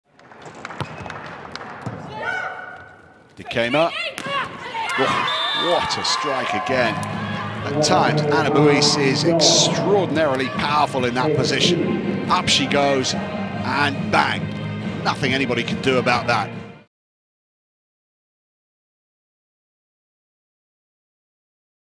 Don't mess with Anne Buijs
Anne Buijs of the Netherlands overpowers the Argentinian defence with a rocket-like spike.
Quick links: FIVB Volleyball Nations League